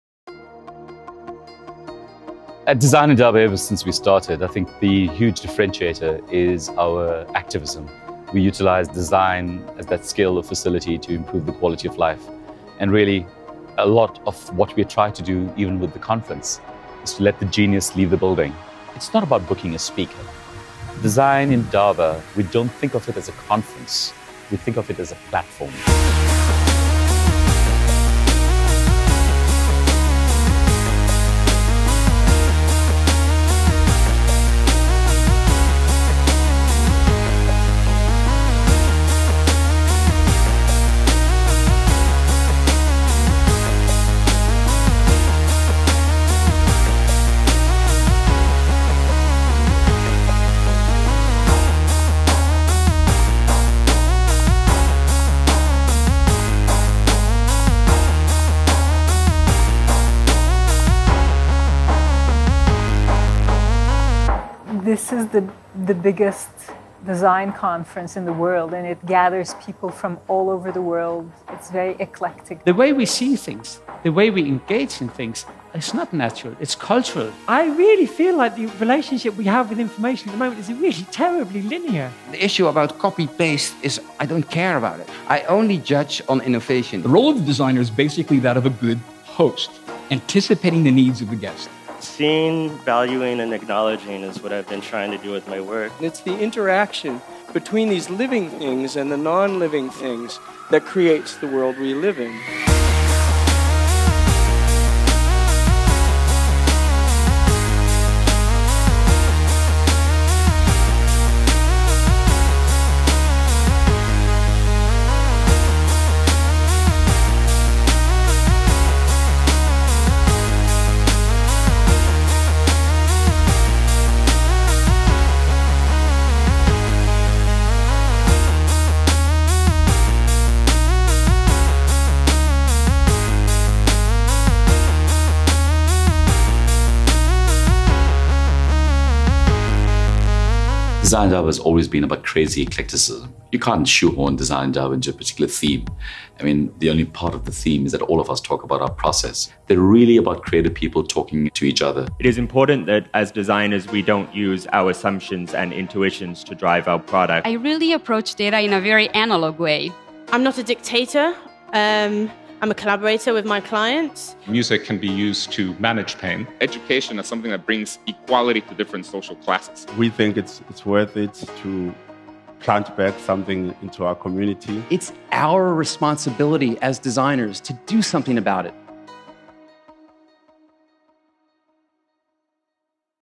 Design Indaba Festival 2017
All arenas of the Design Indaba Festival 2017 came together in one immersive, highly sensorial, creative experience. For the first time this year, we a creative director to lead the conception and building of an Afro-centric dreamscape. In addition to the talks you’ve come to know and love, the Artscape was filled with activations, installations, exhilarating performances and beyond.